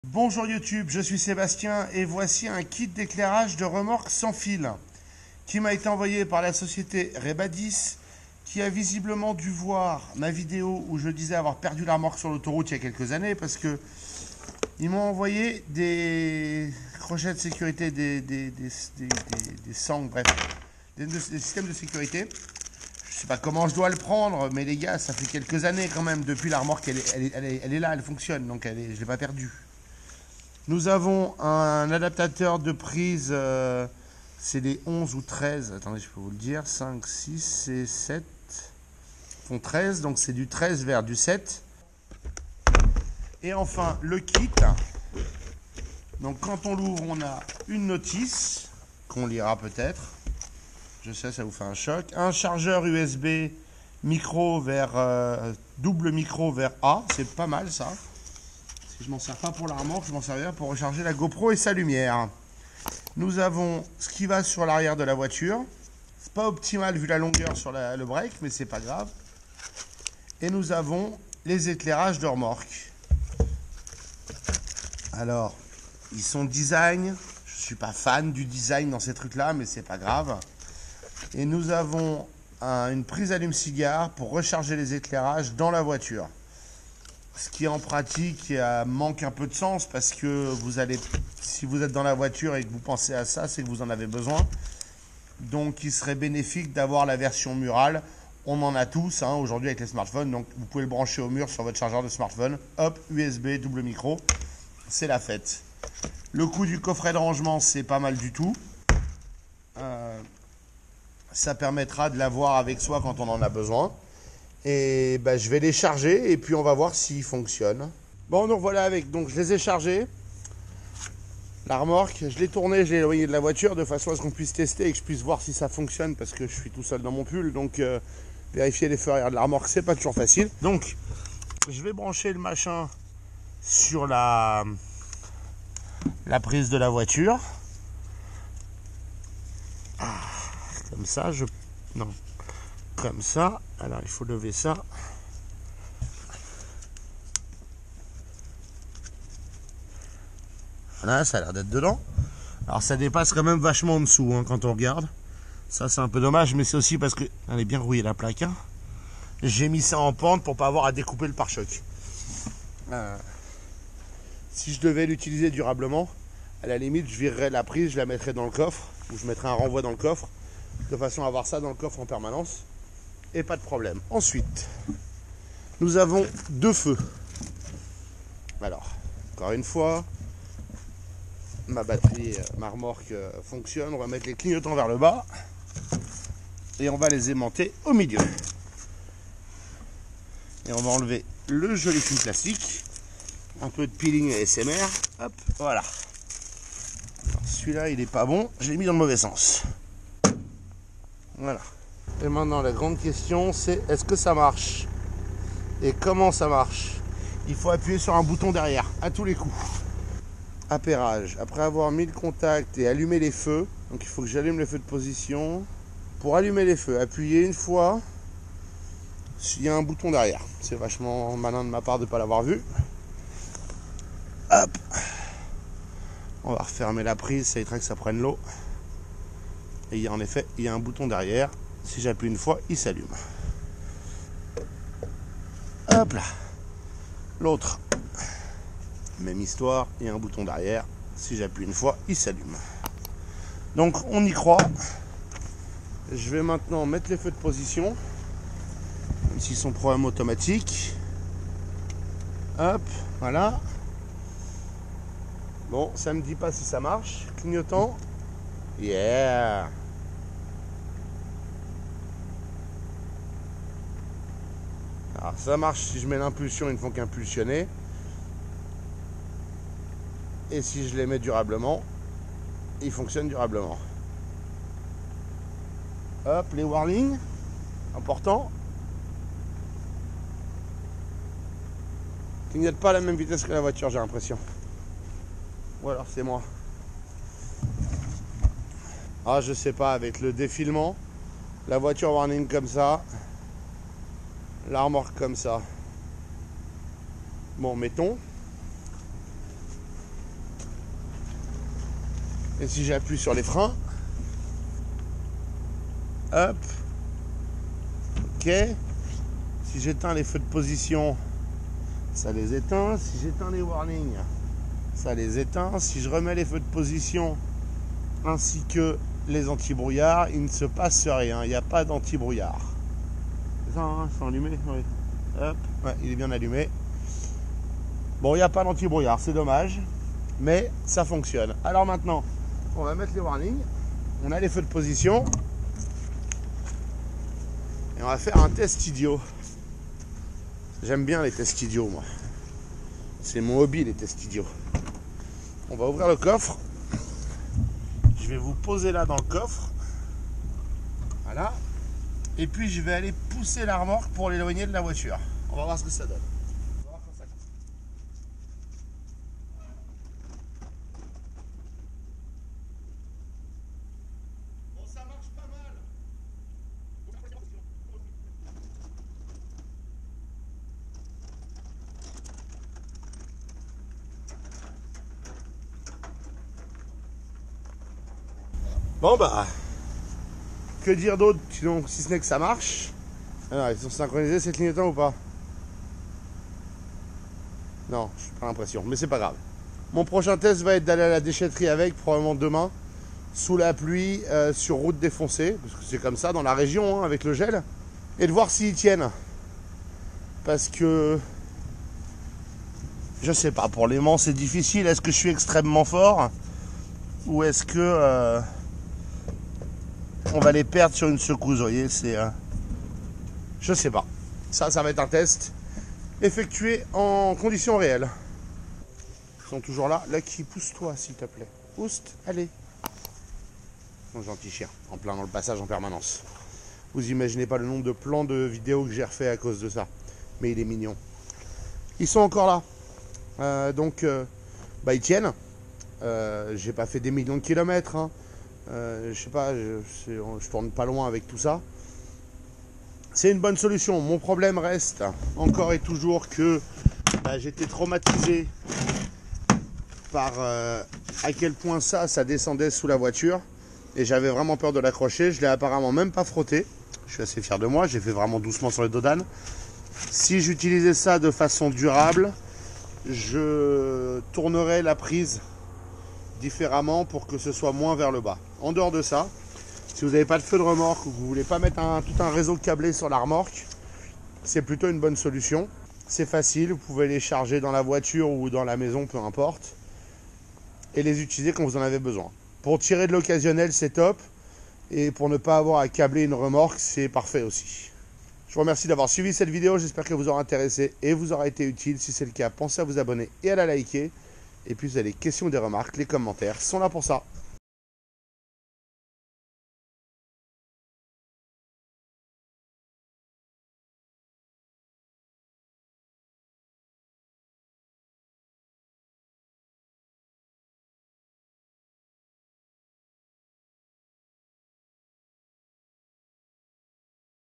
J'ai une remorque sans fil !!!!!
Oui les sangles métalliques servent à accrocher les feux à la remorque pour éviter qu'ils ne tombent et soient perdus. Non je ne les ai pas utilisées et ne les ai toujours pas perdus malgré de routes en mauvais états et de nombreuses visites à la carrière.

Ces feux de remorque sans fil sont très pratiques, surtout si vous avez une remorque sans faisceau électrique ou un faisceau électrique en panne ou défectueux. Cela permettrait également de ne pas installer la prise à l'extérieur de la voiture mais cachée dans le coffre et ainsi aucun risque de corrosion.

Si vous voulez acheter ces feux de remorque sans fil, suivez ce lien